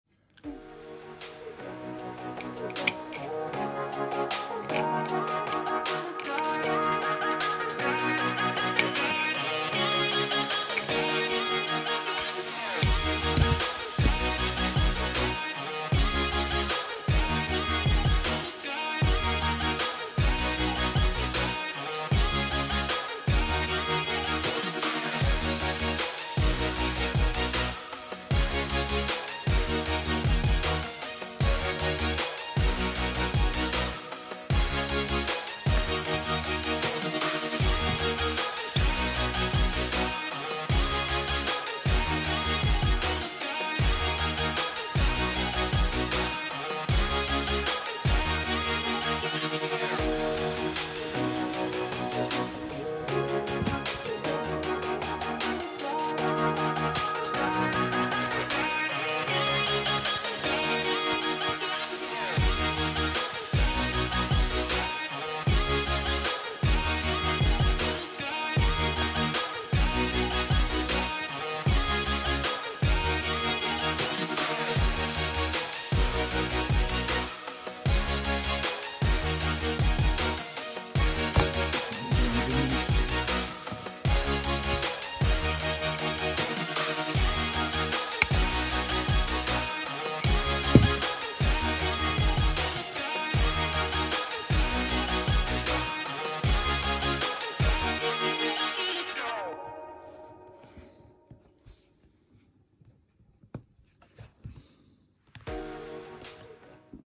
How we make metal ribbon bar pins.
Just short video showing manufacturing of metal ribbon bars. To finish the process the bars are clamped in to shape with a special machine for fantastic finish if you require ribbon bars please head over